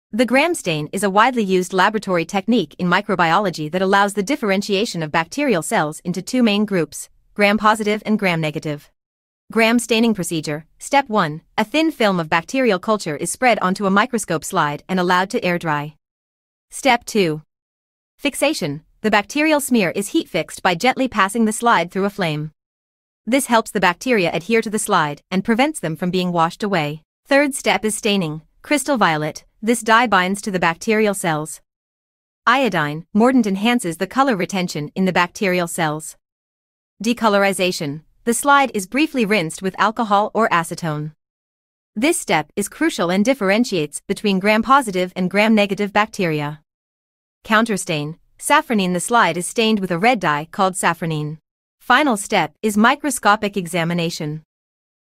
Gram stain test
The Gram stain is a widely used laboratory technique in microbiology that allows the differentiation of bacterial cells into two main groups: Gram-positive and Gram-negative. This staining method was developed by Danish microbiologist Hans Christian Gram in the late 19th century and has become a fundamental tool for classifying and identifying bacteria based on their cell wall properties. mls mlt biology microbiology bacteria gramstaining gram-positive gram-negative